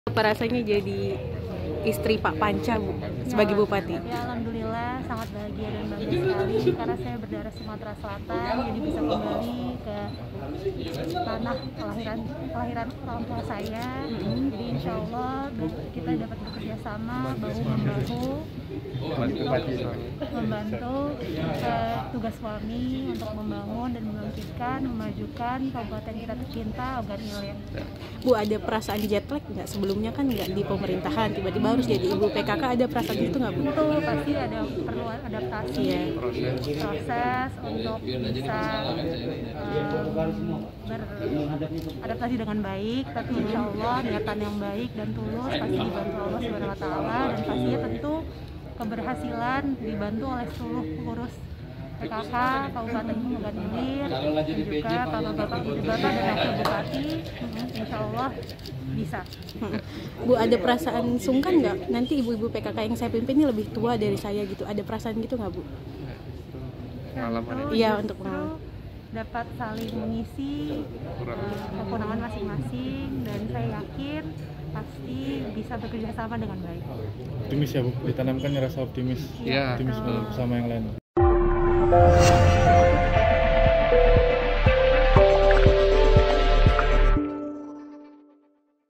Dari Orang Biasa Kini Jadi Istri Bupati, Mikhailia Tikha Alamsjah Siap Adaptasi Demi Dampingi Panca
SRIPOKU.COM, PALEMBANG - Mikhailia Tikha Alamsjah turut menghadiri pelantikan enam kepala daerah di Sumsel terpilih di Griya Agung Palembang, Jumat (26/2/2021).

Terhitung sejak pelantikan tersebut, Mikhailia resmi memiliki seorang suami berstatuskan bupati, tepatnya Bupati Ogan Ilir.

Dari orang biasa kini mendampingi seorang Panca Wijaya Akbar menjalankan tugas sebagai bupati, Mikhailia mengaku siap untuk beradaptasi.